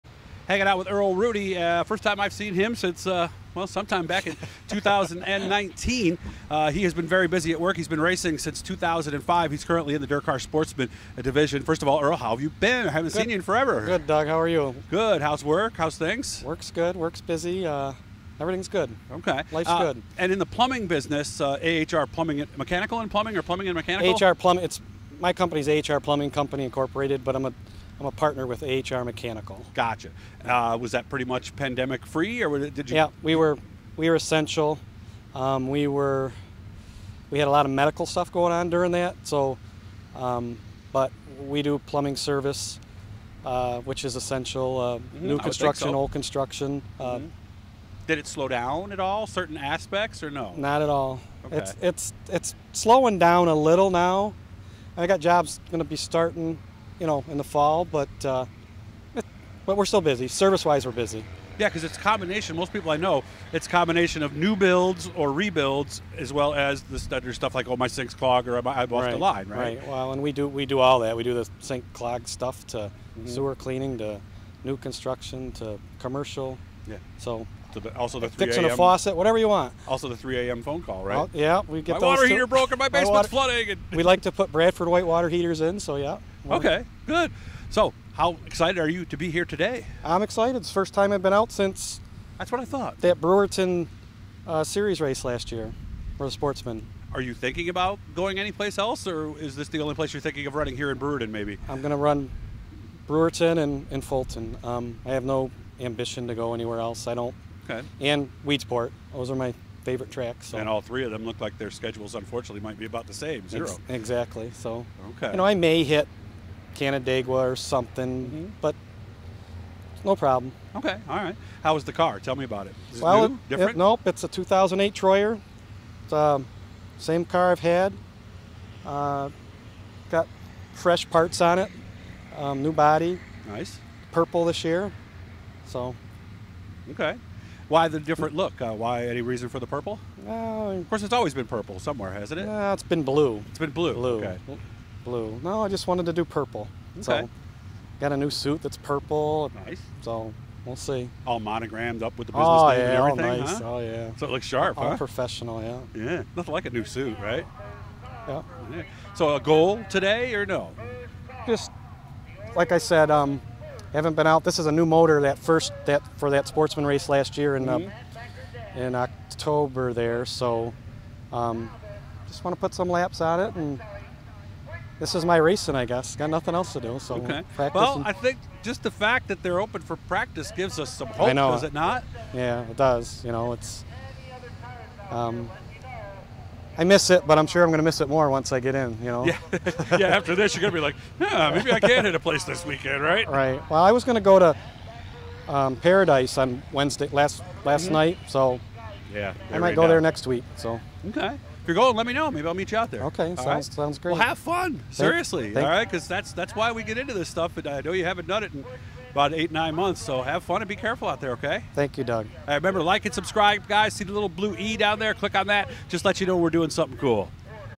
Earl Rudy at Fulton Speedway Practice Jul 23 2020
Earl Rudy talked about his hopes to get back to Brewerton and Fulton Speedway as he got ready for some practice laps.